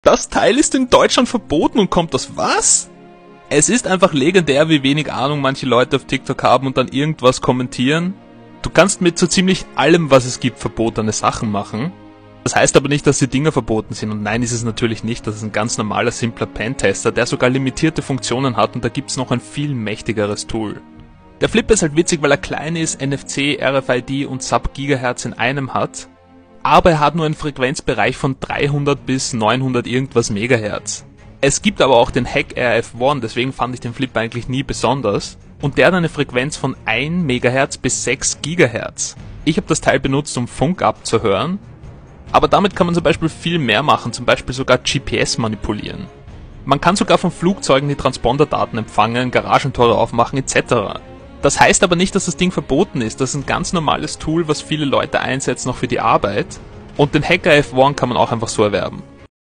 Flipper Zero Vs. Nintendo Switch
Here we have a video showing off the Flipper Zero & its multiple capabilities.